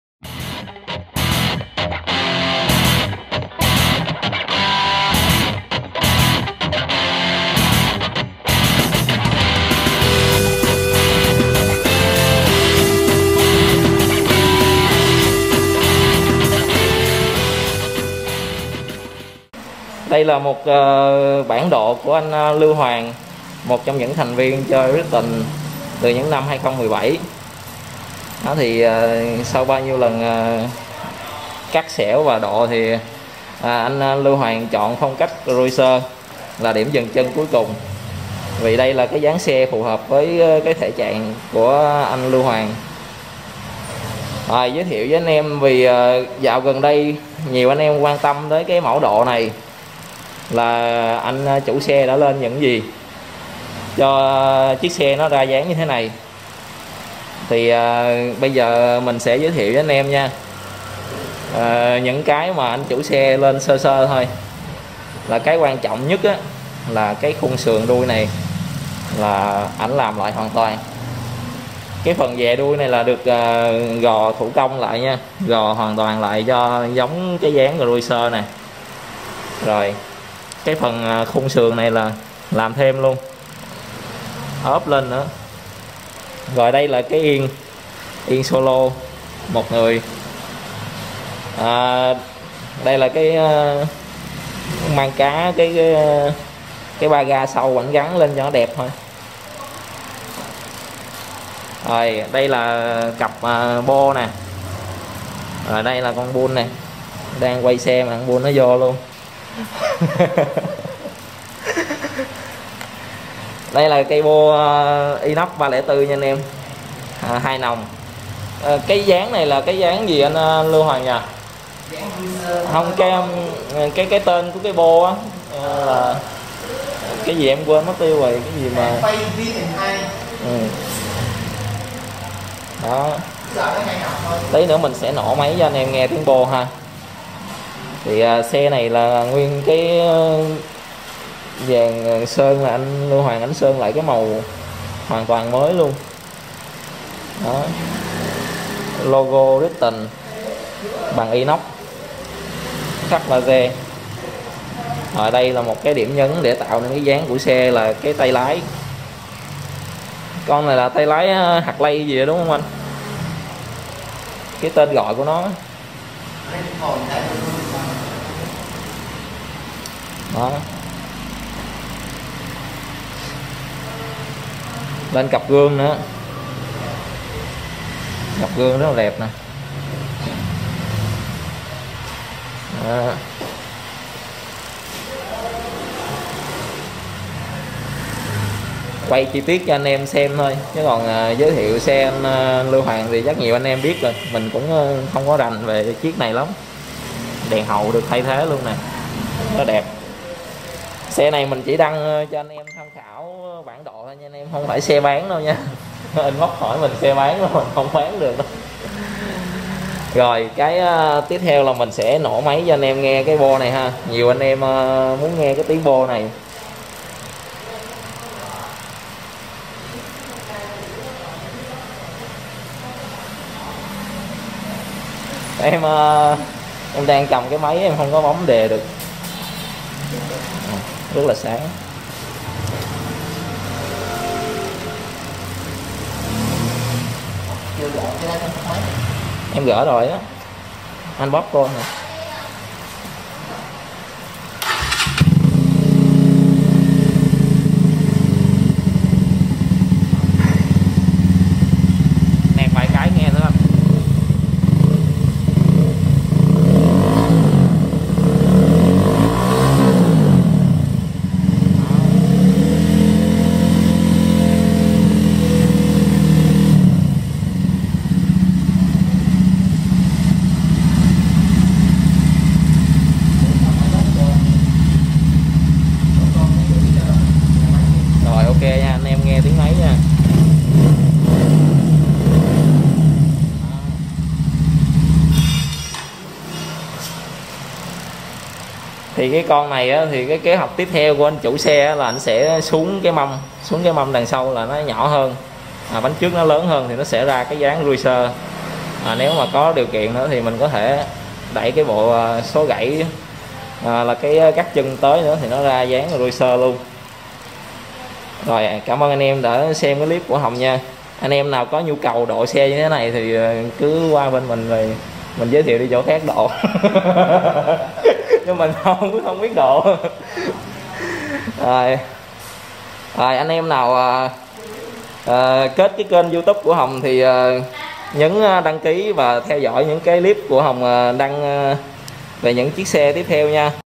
Brixton up Cruiser bao ngầu nha ae
Một trong những mẫu độ đẹp dáng cruiser cho Brixton..!
Ae nào muốn lên như vậy ghé bên mình nhé
MT_Motorcycle 27 Nguyễn Du, P7, Gò Vấp